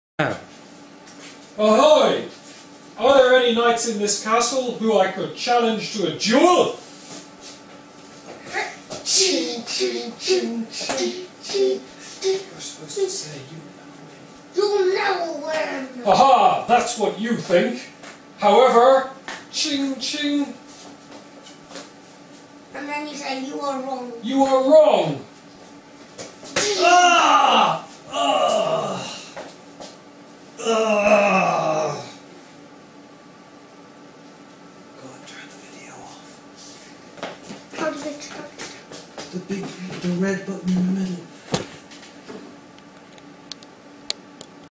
Duel
Donnie and dad having a duel